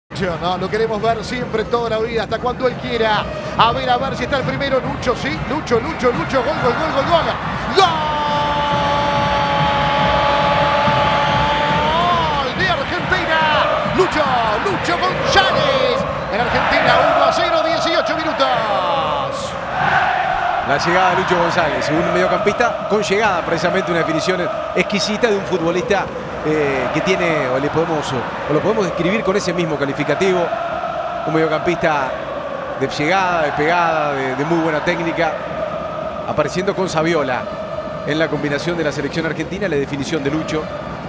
Gol de Lucho González - Selección 1-0 Boca - Un partido para toda la vida
Conexión riverplatense en el primer gol del partido: Saviola para Lucho González y el 1 a 0 de la Selección Argentina contra Boca